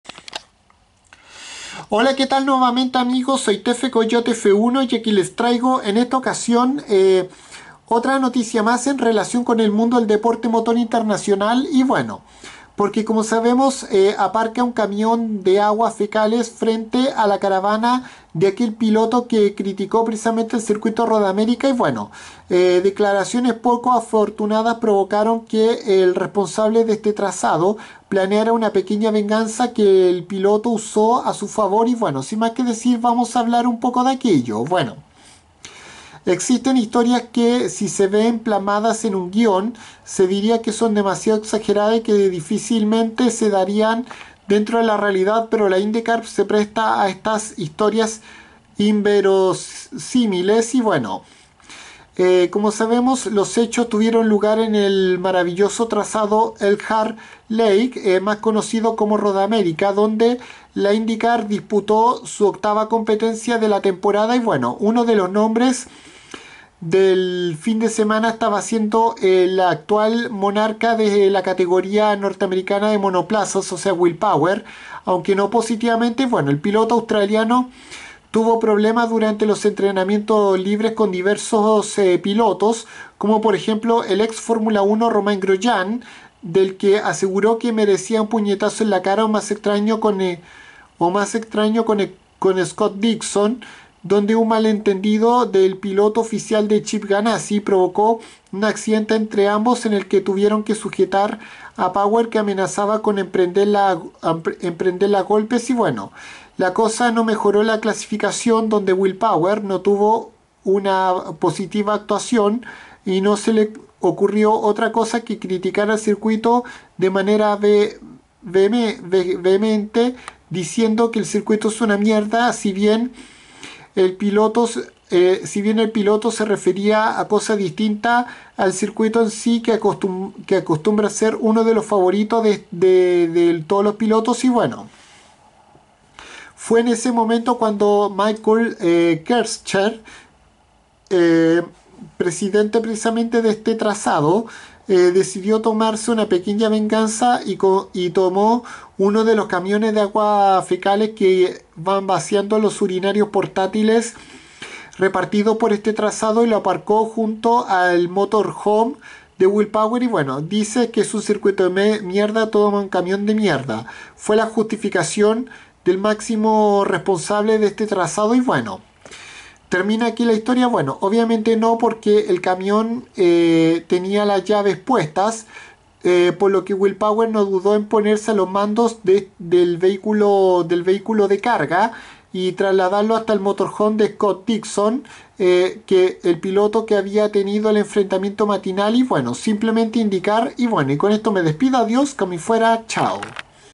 Aparcan un camión de aguas fecales frente al motorhome del piloto que criticó al Road America
Esto ocurrió precisamente en la última competencia de la actual Temporada 2023 de la IndyCar donde como una pequeña venganza el dueño del circuito tomo un camión de líquidos residuales y lo estacionó frente al motorhome del piloto que lo criticó